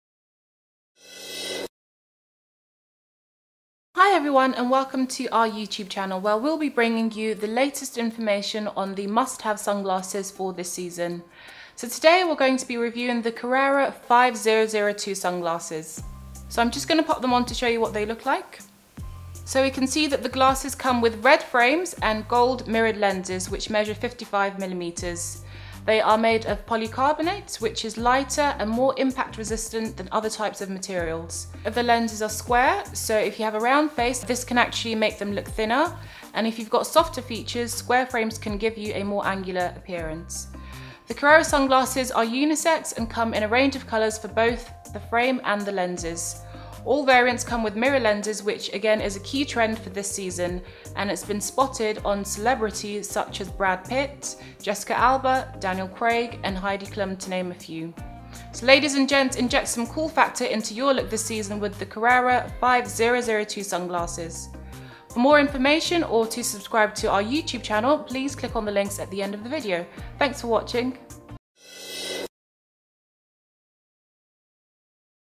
Carrera 5002 Sunglasses Review | VisionDirectAU
This model in this video comes with red frames and gold mirrored lenses which measure 55mm. They are made of polycarbonate which is lighter and more impact resistant than other types of materials.  The shape of the lenses is square so if you have a round face, this can help make them look thinner. If you’ve got softer features, square frames can give you a more angular appearance.
The Carrera 5002 sunglasses are unisex and come in a range of colours for both the frame and lenses. All variants come with mirrored lenses which is a key trend this season and has been spotted on celebrities such as Brad Pitt, Jessica Alba, Daniel Craig and Heidi Klum to name a few. Inject some cool factor into your look this season with the Carrera 5002 sunglasses.